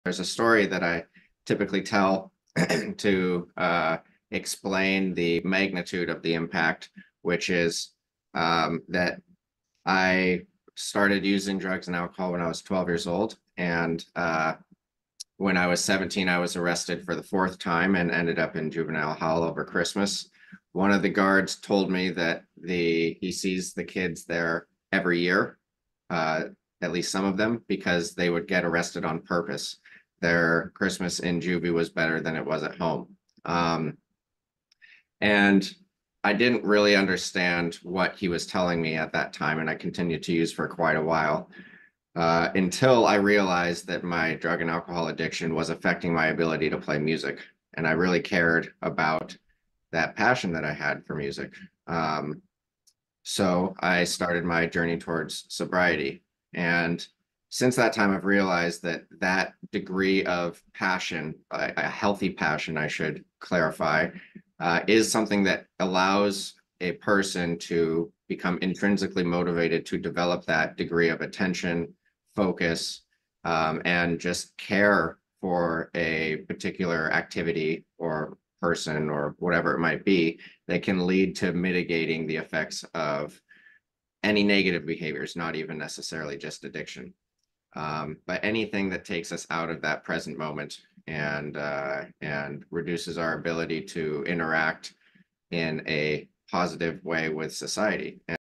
Unlocking Passion: Overcoming Addiction and Finding Purpose
Discover the transformative power of passion in this inspiring story of overcoming addiction and pursuing a fulfilling life. Join us as we explore the journey towards sobriety and the positive impact it can have on personal growth.